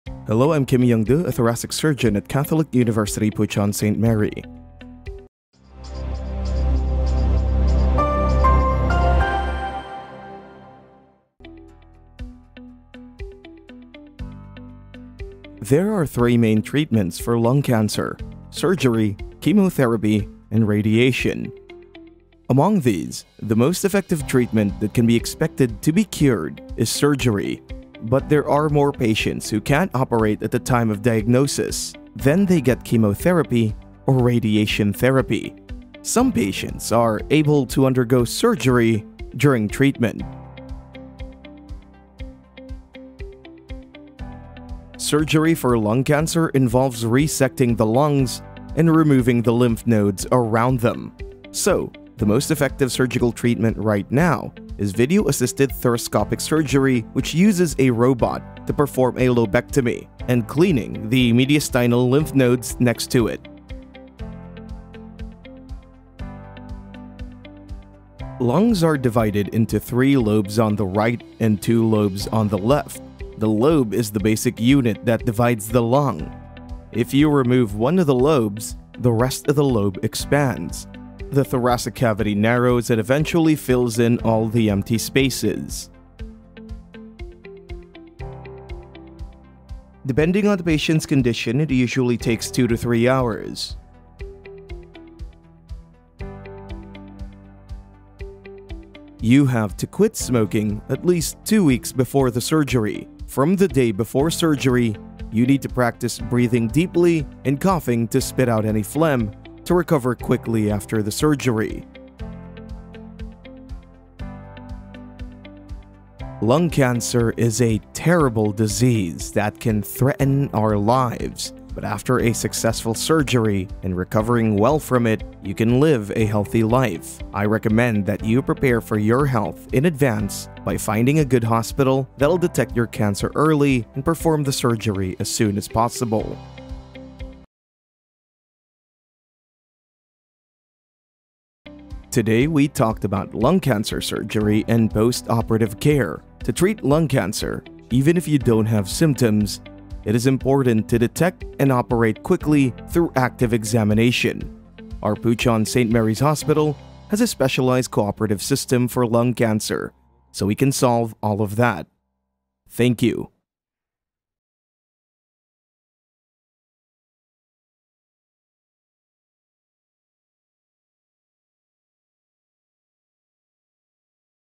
Lung cancer surgery and treatment
Hello! In this video, we will be discussing lung cancer surgery and other treatment options.

Surgery is one of the treatment methods for lung cancer with the highest survival rates.

In this video, we will explain other treatment methods besides surgery and how the surgery is performed.

If you are struggling with lung cancer, we hope you find this video informative and helpful.

Interview with Dr. Kim (from Bucheon St. Mary's Hospital)